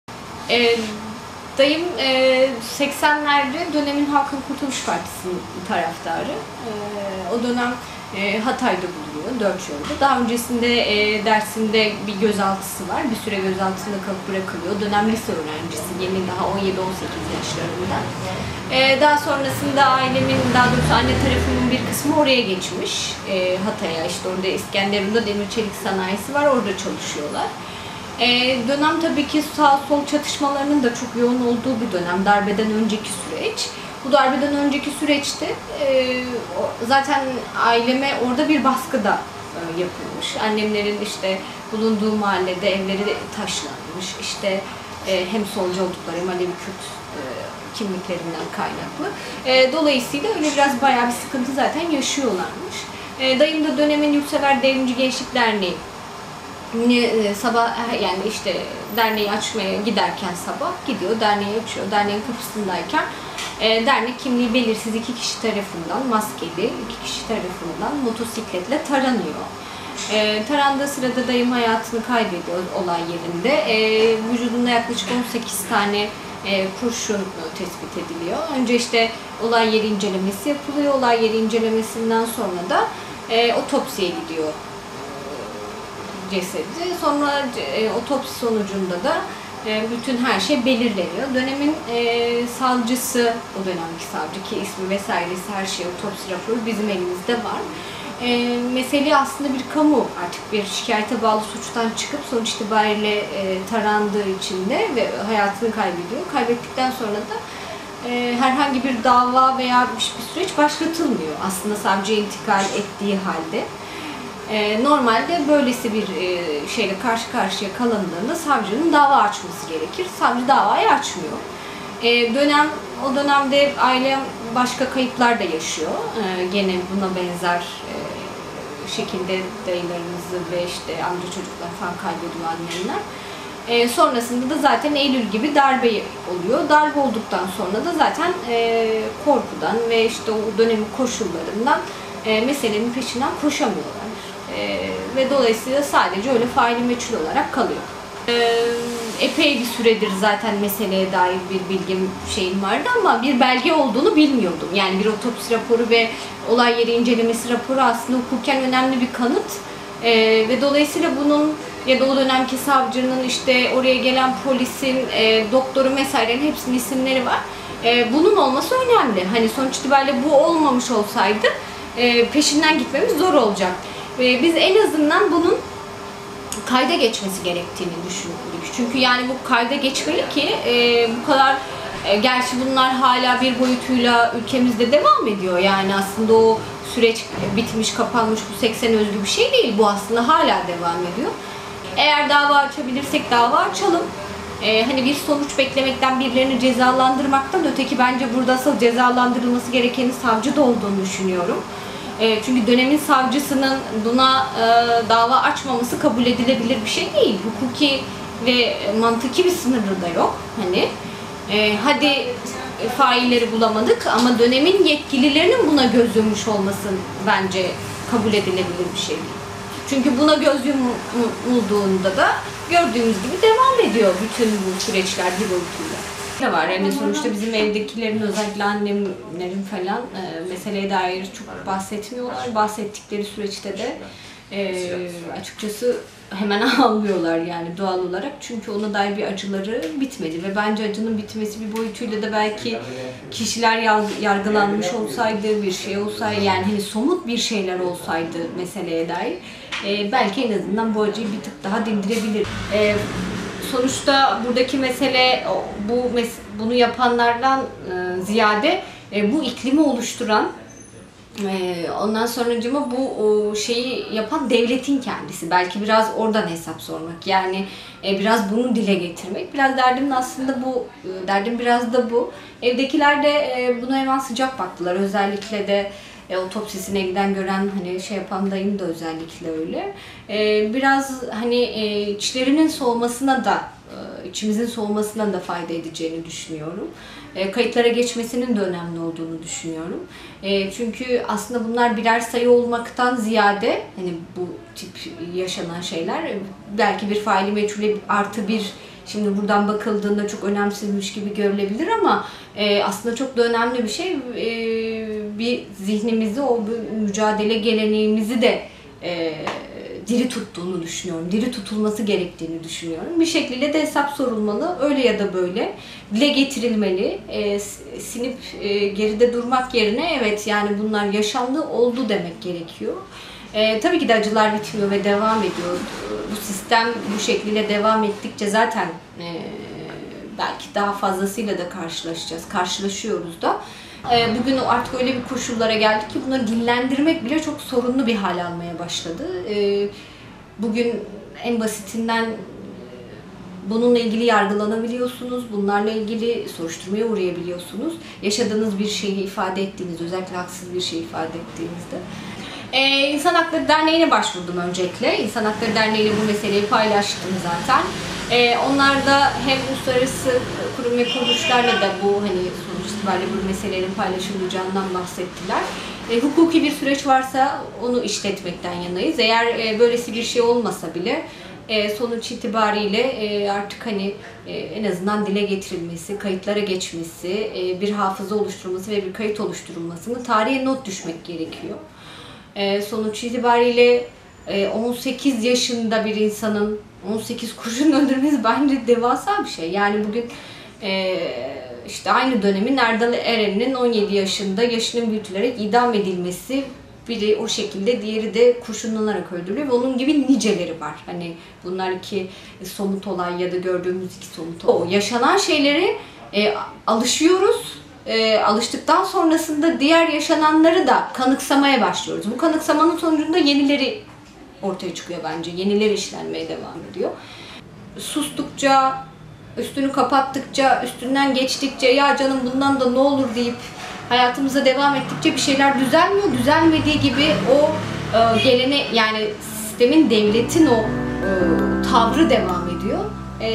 Faili meçhule kalmaması için 40 yıl sonra ilk başvuru
Hatay’ın Dörtyol ilçesinde 1980’de üyesi olduğu YDGD’yi açarken 18 kurşunla öldürülen 18 yaşındaki Düzgün Yıldız’ın yeğeni Gamze Yentür, 40 yıl önce işlenmiş cinayetin faili meçhul kalmaması için İHD’ye başvurdu.

Hatay’ın Dörtyol ilçesinde 20 Ocak 1980 tarihinde dönemin Yurtsever Devrimci Gençlik Derneği’nin (YDGD) üyesi Düzgün Yıldız (18), derneği açtığı sırada maskeli iki kişi tarafından silahlı saldırıya uğradı. Saldırıda 18 kurşunla yaşamını yitiren Yıldız, olay yeri incelemesi ve otopsi işlemlerin ardından cenazesi ağabeyi Haydar Yıldız’a teslim edildi. Yıldız, ailesi tarafından Dersim’de defin edilirken, o dönemde aile içinde bulunan birçok akrabası da faili meçhul olaylarında katledildi. Olay yeri incelemesi ve otopsi raporlarında imzası olan dönemin savcı vekili Hanifi Tiryaki'i tarafından herhangi bir soruşturma yürütülmedi ve davaya dönüştürülmedi. Yıldız ailesi de o dönem üzerlerindeki devlet baskısından dolayı herhangi bir başvuruda bulunmadı. 40 yılın ardından İnsan Hakları Derneği (İHD) İzmir Şubesine başvuruda bulunan Yıldız’ın yeğeni Gamze Yentür, başlattıkları süreci Mezopotamya Ajansı’na (MA) anlattı.